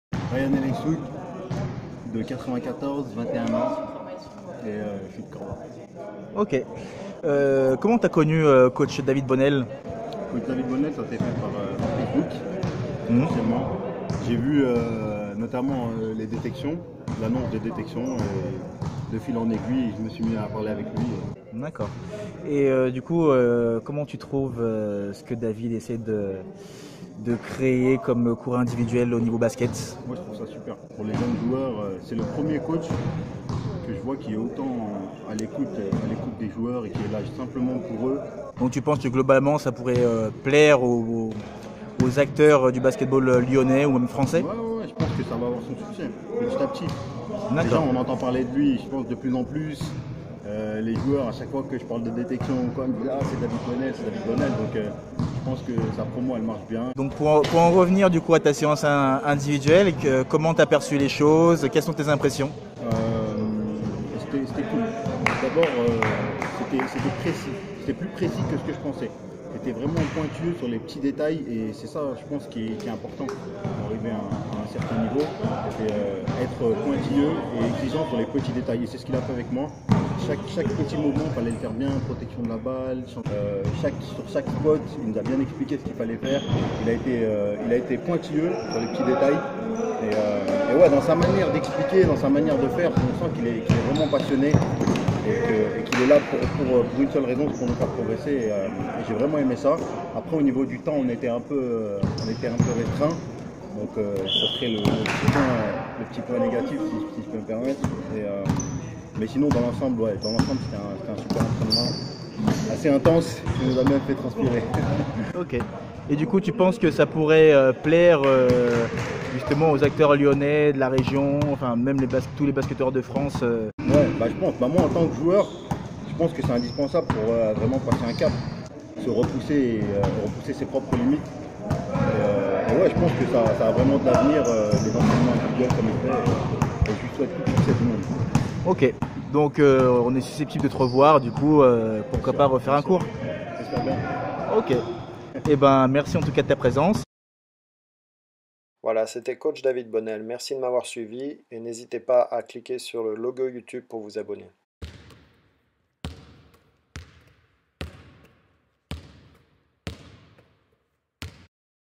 ITW seance basket individuel Rayan Lyon
Le travail de basket individuel est fondamental pour la progression du joueur dans un collectif, toi aussi tu veux progresser ?
ITW séance basket individuel Rayan Lyon